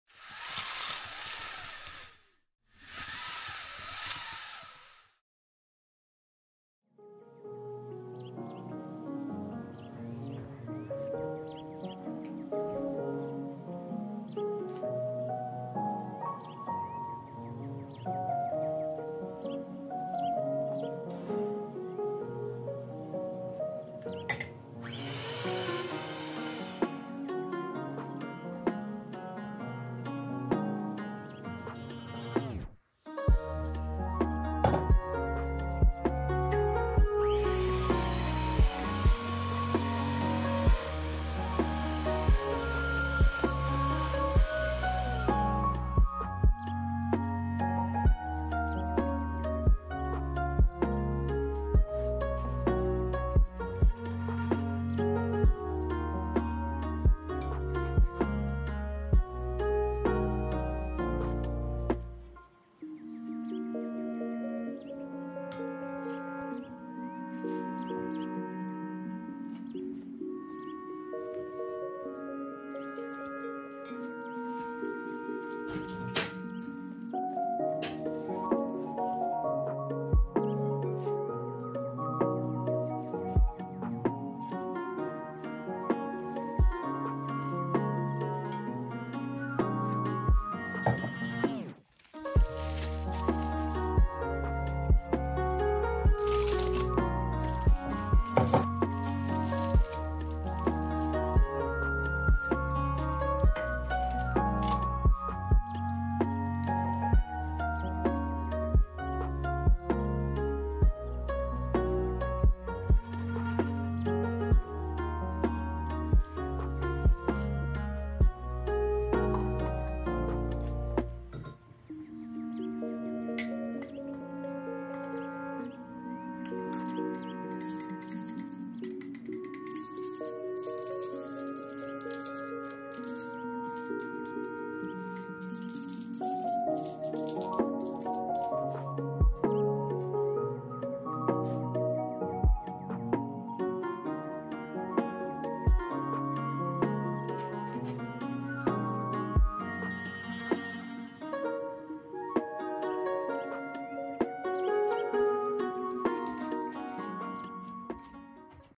Savoyardi homemade cookie recipe. An easy Savoyardi recipe for your Tiramisu
Ingredients:

• 3 eggs
• 100 g sugar
• 75 g flour
• 1 g salt
• 30 g powdered sugar